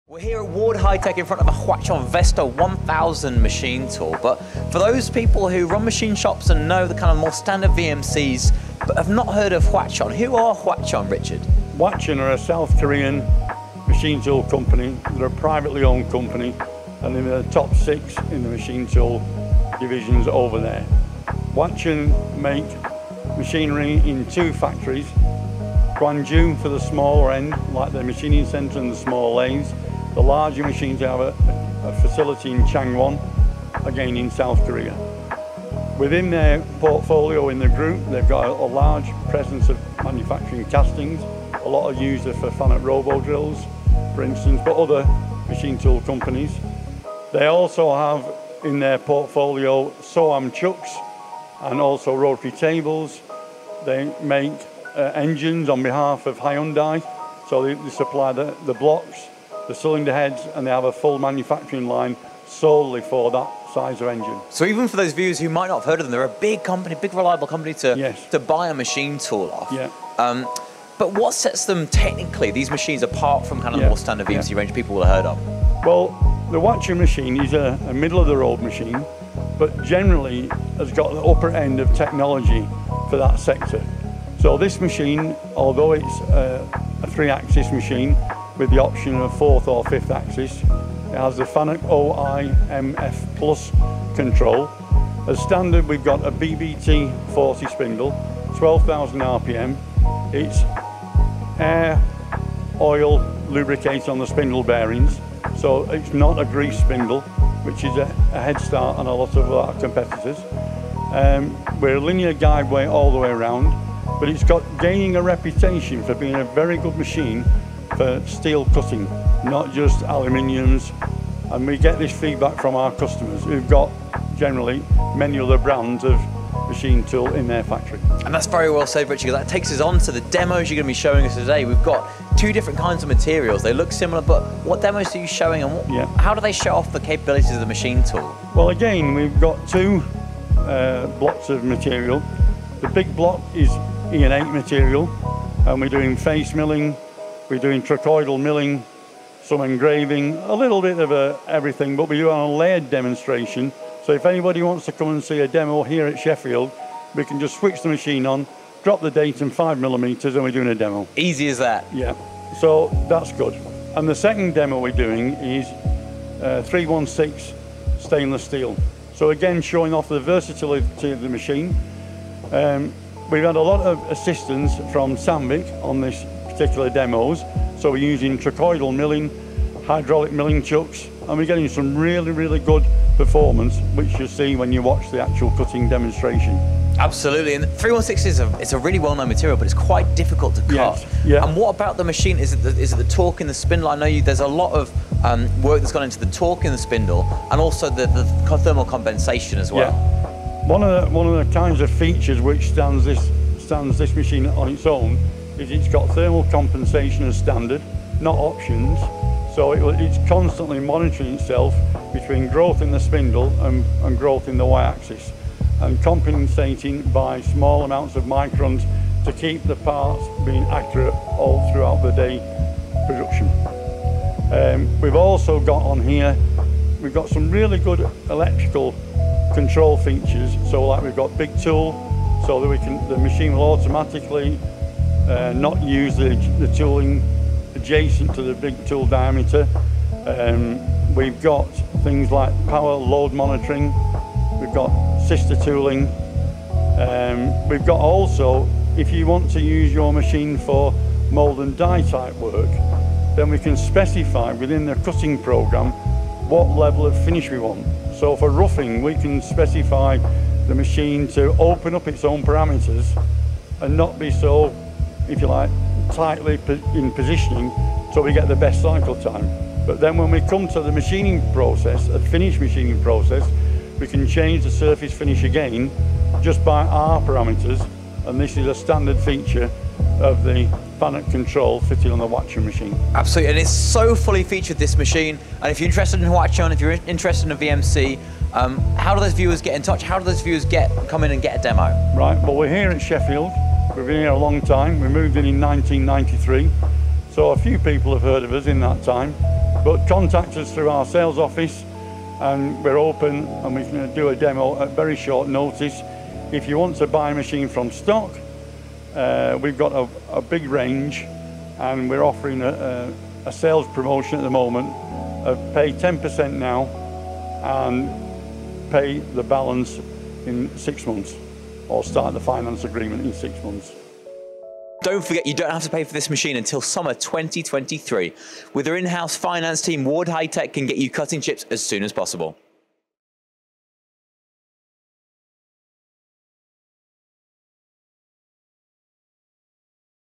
Is this one of the cleverest vertical machining centres on the market?
In this video watch the VESTA VMC range from Hwacheon cut two differing demonstrations both of which you can see for yourself at short notice by visiting Ward Hi Tech's showroom in Sheffield. Plenty of technology on show and discussed during in the video. It's great how this machine can quickly adapt its speeds and feeds depending on the operation being performed. Therefore if your roughing, you can be assured of maximising metal removal and if you're finishing you'll get the best surface finish whilst achieving tight tolerances, this is just one of many features discussed with Richard.